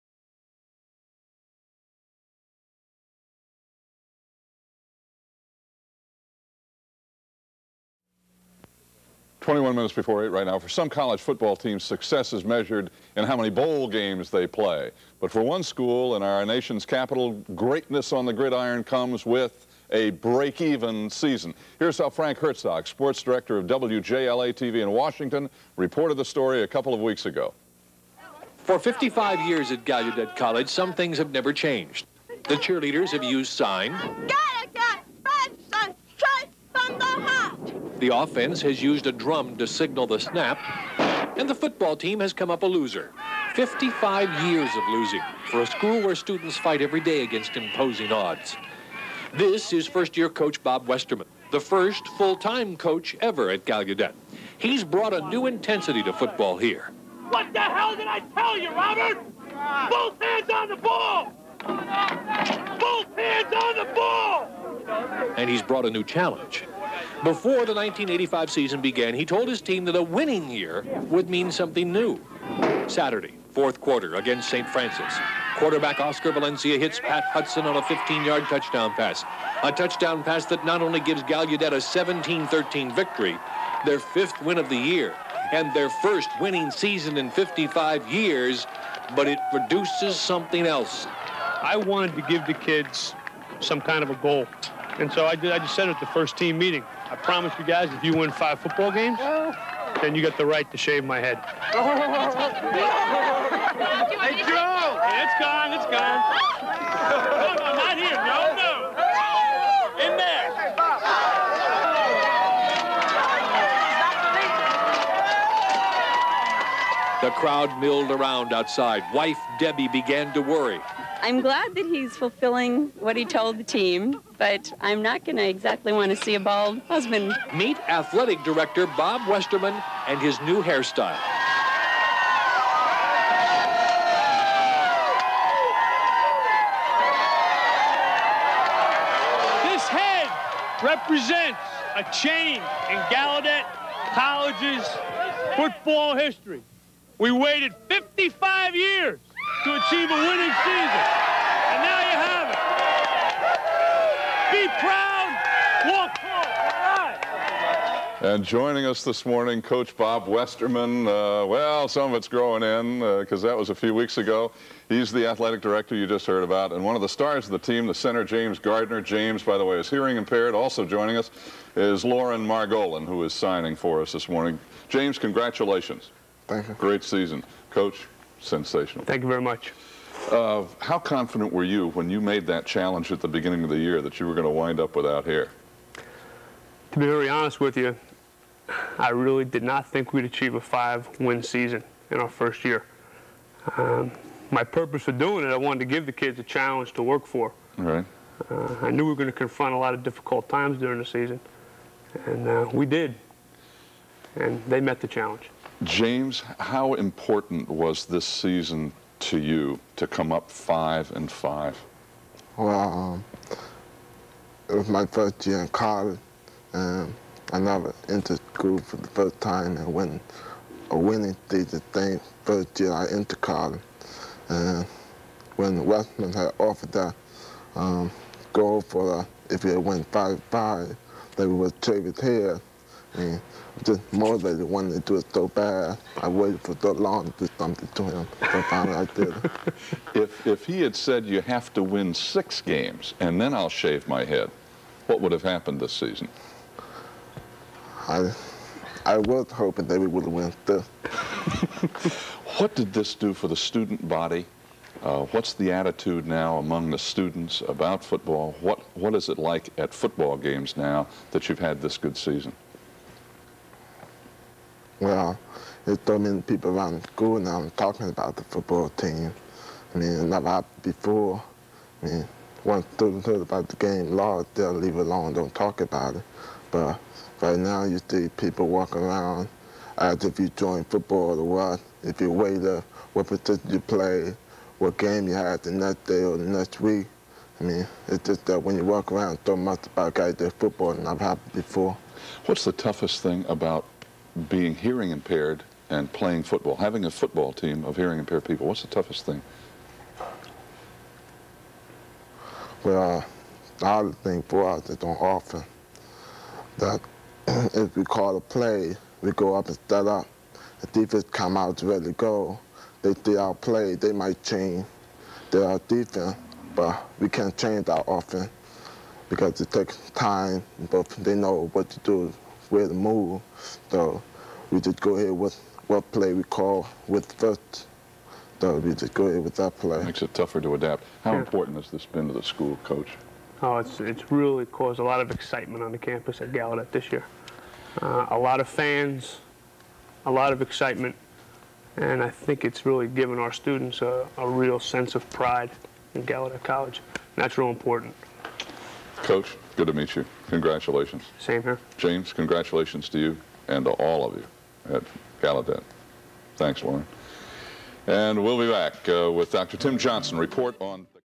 Gallaudet Bison Football Team goes 5-5: Coach Westermann Goes Bald!
Fall of 1985